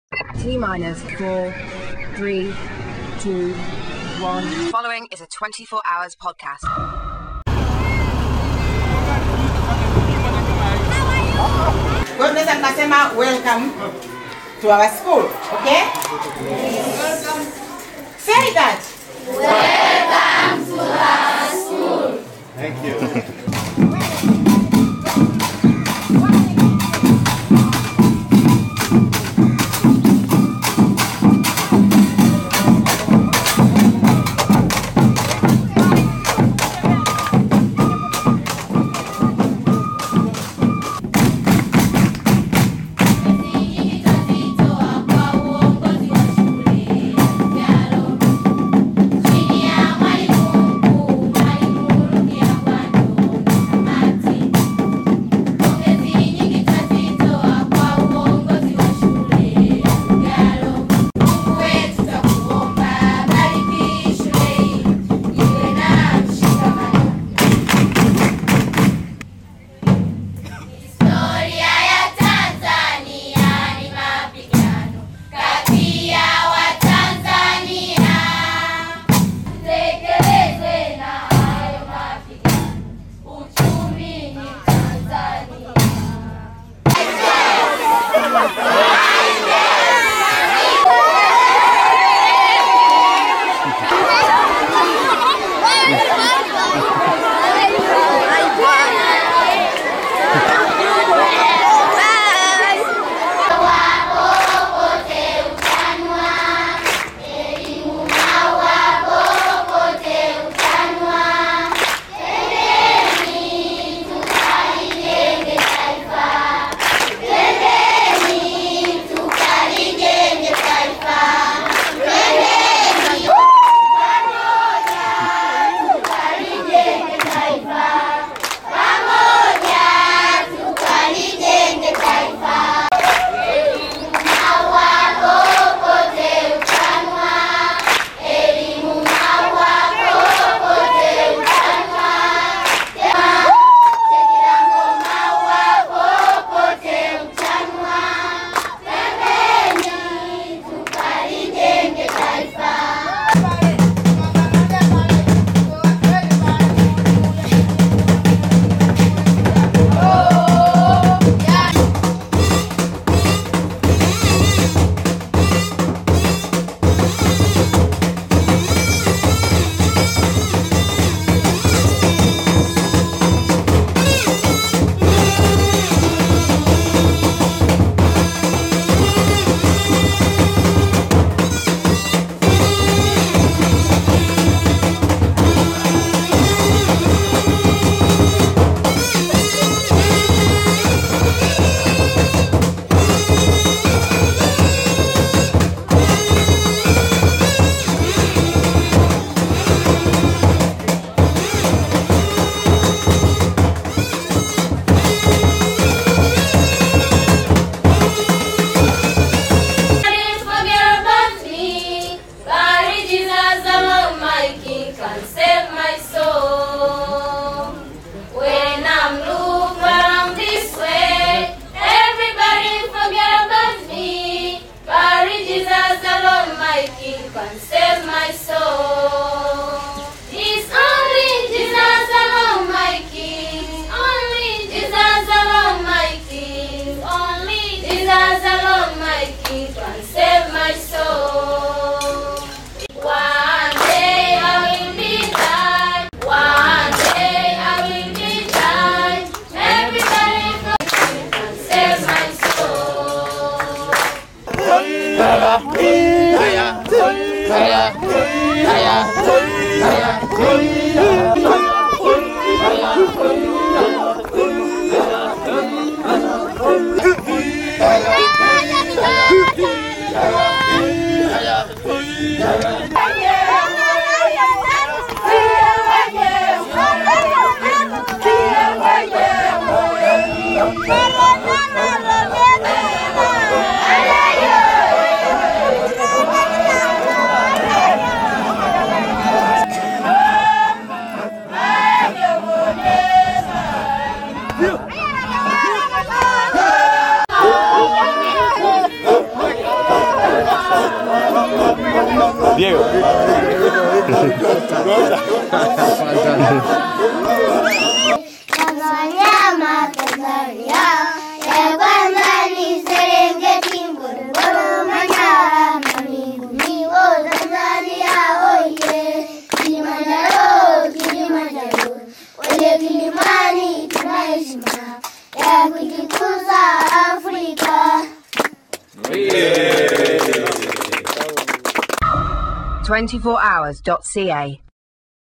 Whitecaps welcomed with open arms and song in Tanzania
The Vancouver Whitecaps were met with a musical welcome wherever they went in Tanzania during their historic soccer safari. Here are highlights of the school and community performances during the March 6-18, 2009 tour by the United Soccer Leagues' First Division champions.